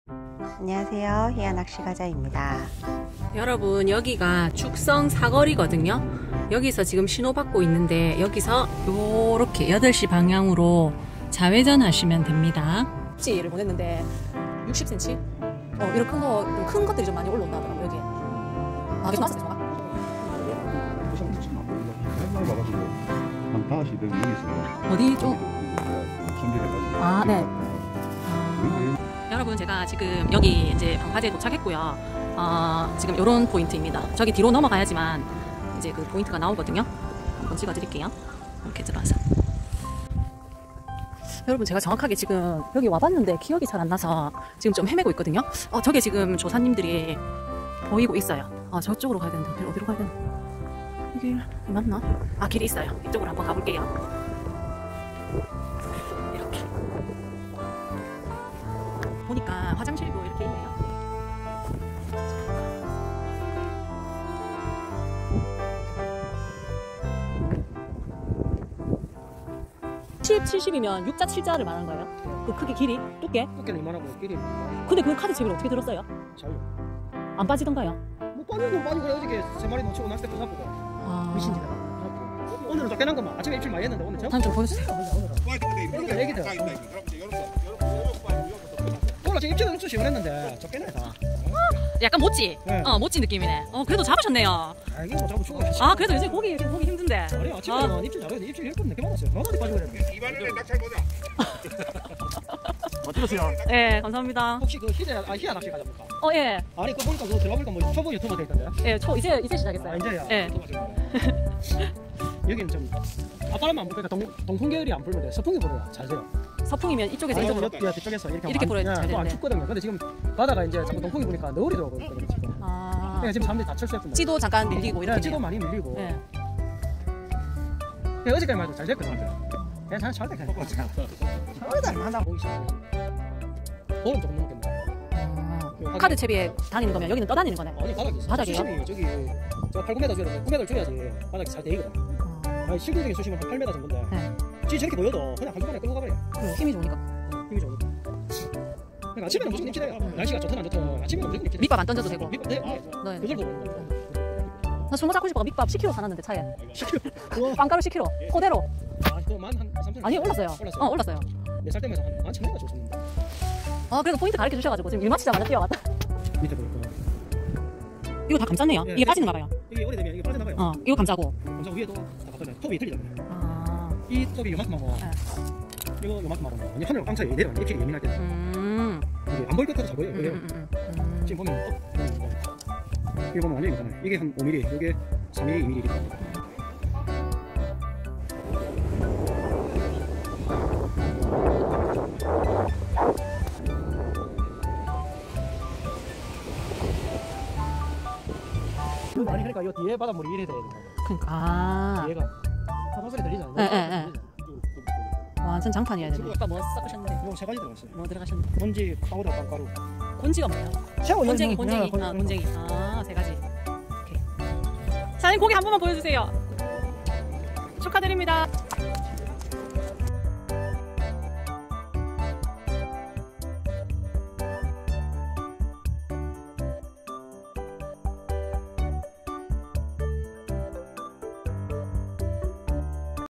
모치부터 대숭어까지 올라오는 포인트!^^ 외항(테트라포인트)내항(발판좋아요)
주소ㅡ죽성로(264)
입니다~^^
보통 죽성외항 테트라포인트라고 하면 아신다고 합니다!